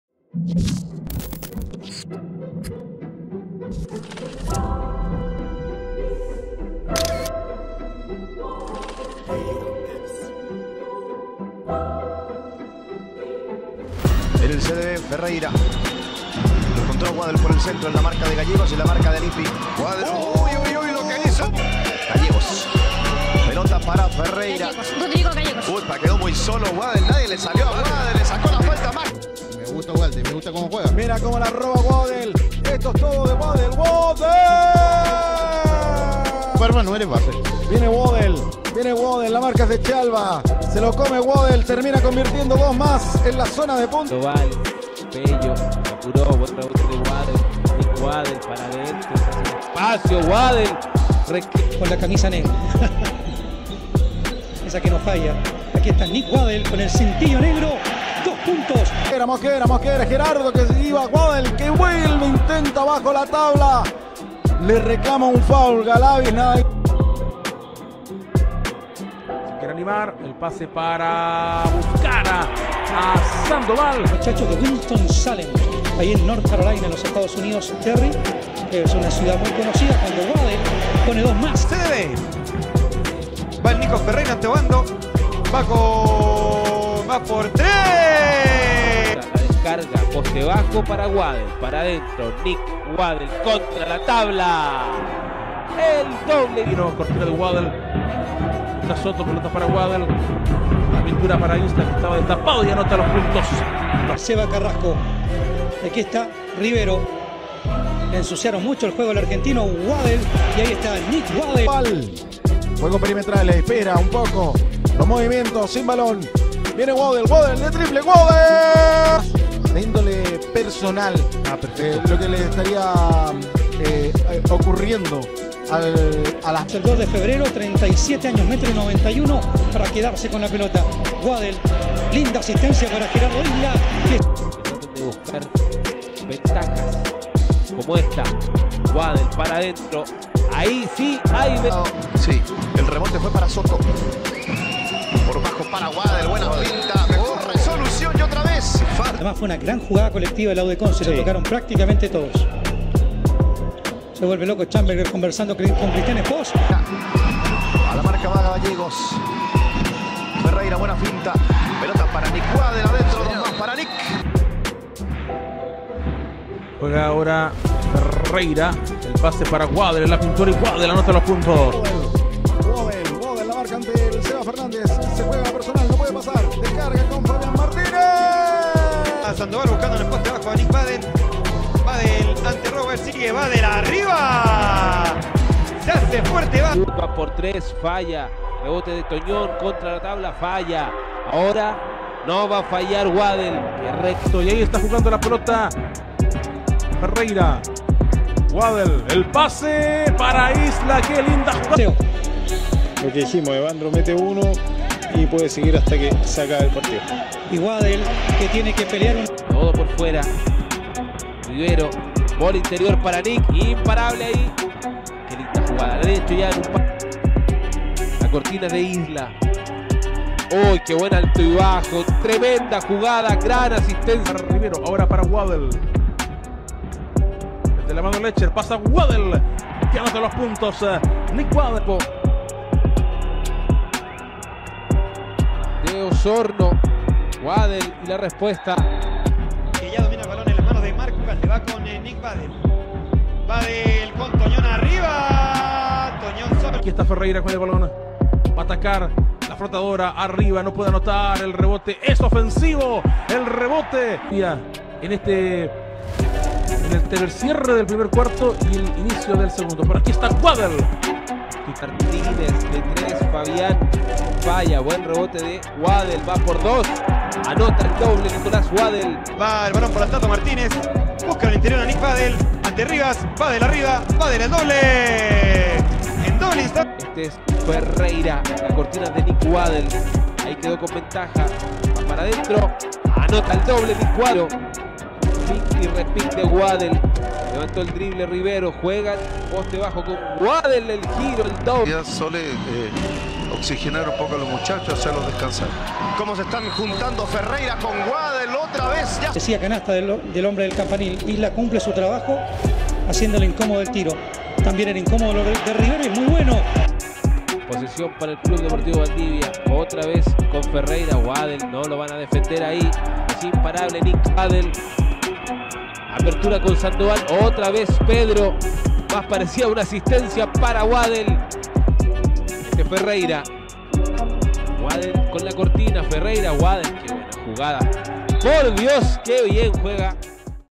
Nicholas Wadell Jr Highlights 2022/23 || Chile LNB || Club Deportivo Valdivia
◉ Nicholas Wadell Jr
◉ Club Deportivo Valdivia
◉ Center, 2.00 m